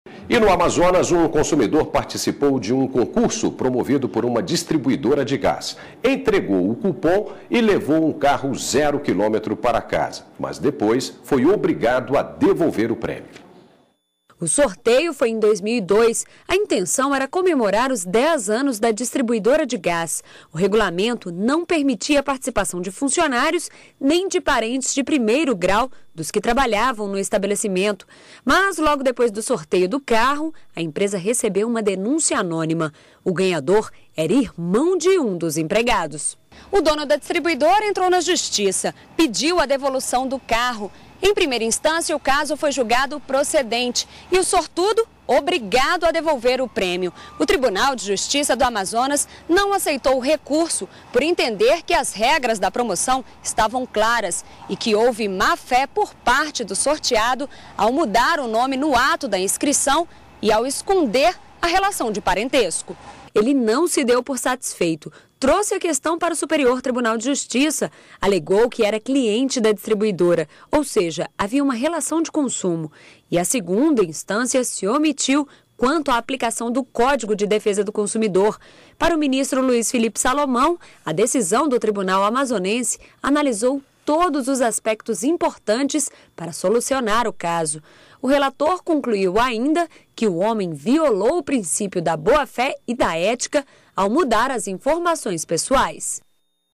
Devolução prêmio (05/10/12)
No Amazonas, um consumidor participou de um concurso promovido por uma distribuidora de gás. Entregou o cupom e levou um carro zero quilômetro para casa. Mas, depois, foi obrigado a devolver o prêmio.